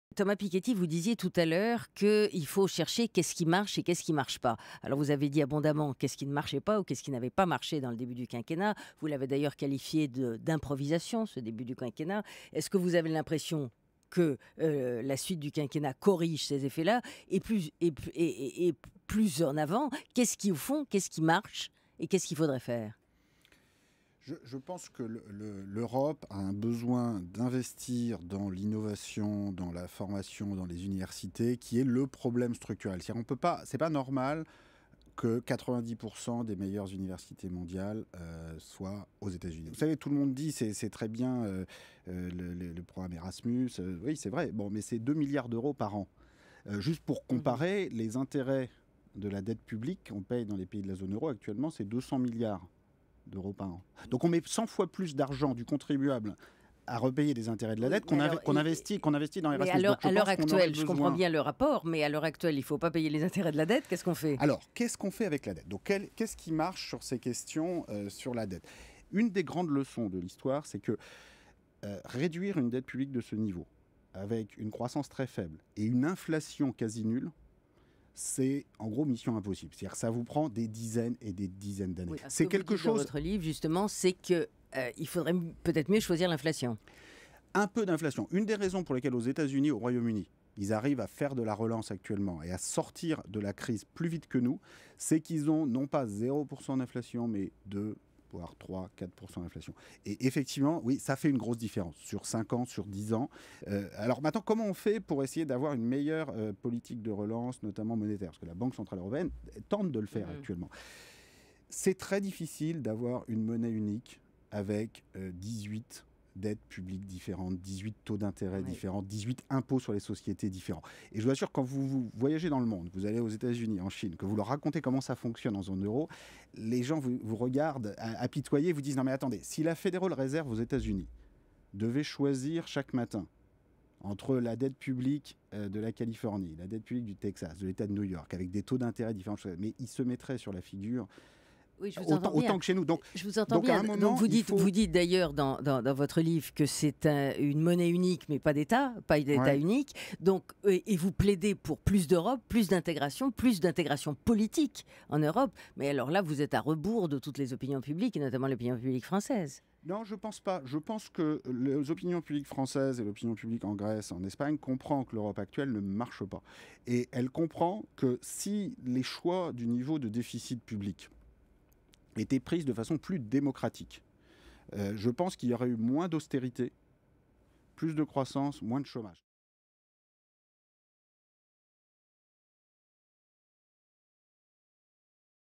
Piketty : "90% des meilleures universités sont aux Etats-Unis"
Pour Thomas Piketty, l'Europe a un besoin urgent d'investir dans l'innovation, dans la formation et dans les universités surtout, sachant que 90% des meilleures universités mondiales sont aux Etats-Unis.